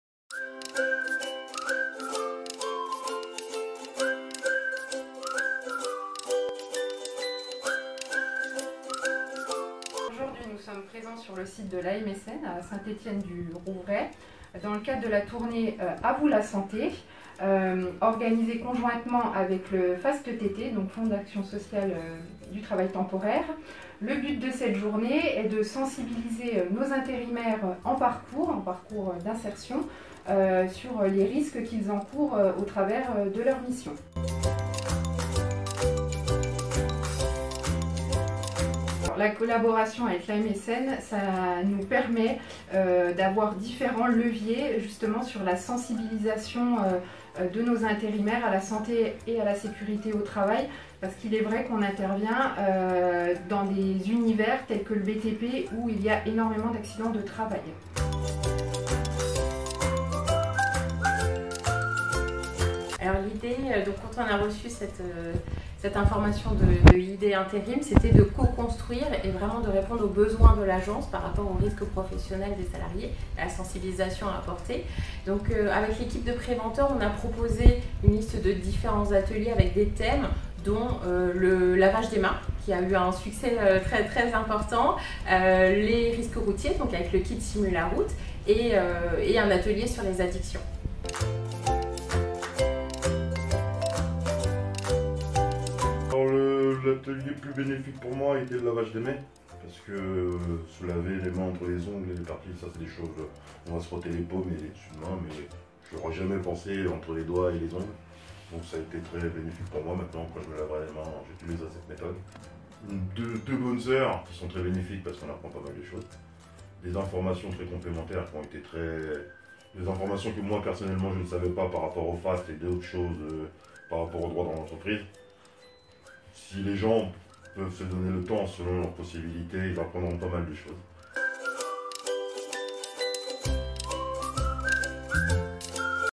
Journée de sensibilisation pour les travailleurs temporaires
Quand une agence d'intérim d'insertion fait appel à son service de santé au travail pour sensibiliser ses intérimaires à la santé au travail, ça donne une journée d'actions autour d'ateliers ludiques et beaucoup de bonne humeur !

Journée réalisée sur le centre AMSN de Saint-Etienne du Rouvray avec le partenariat de :
- Goupe ID'ees Intérim
-FASTT
- AMSN